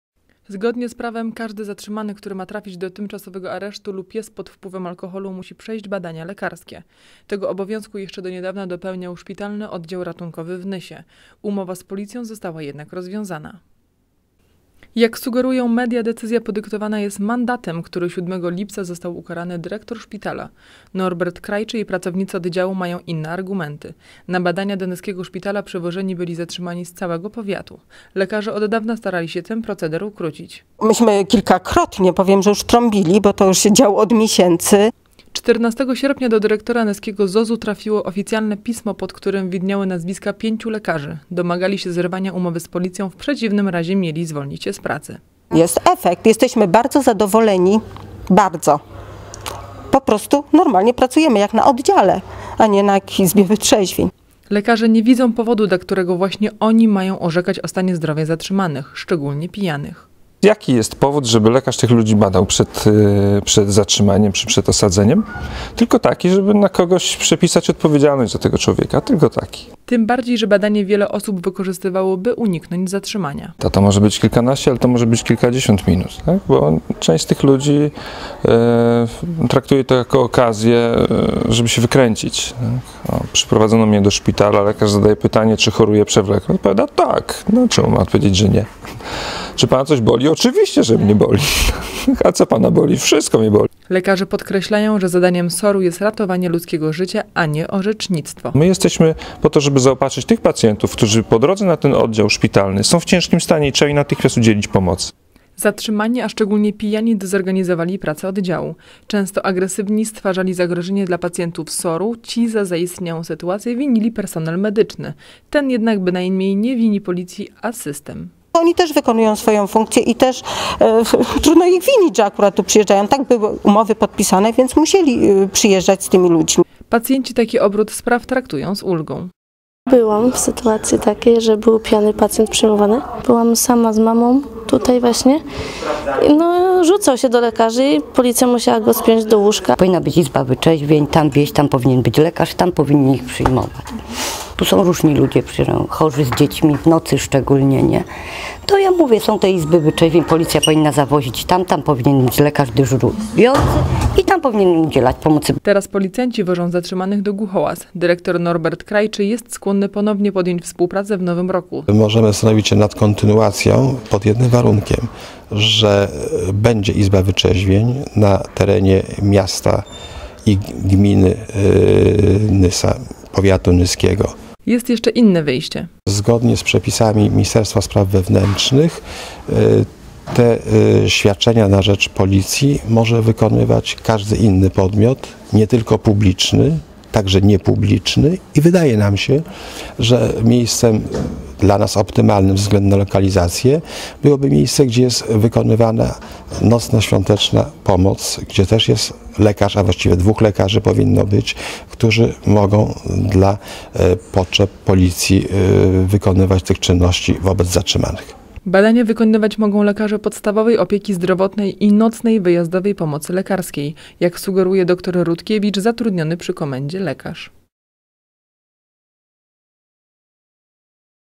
Nietrzeźwi do Głuchołaz
Zgodnie z prawem każdy zatrzymany, który ma trafić do tymczasowego aresztu lub jest pod wpływem alkoholu, musi przejść badania lekarskie. Tego obowiązku jeszcze do niedawna dopełniał Szpitalny Oddział Ratunkowy w Nysie. Umowa z policją została jednak rozwiązana.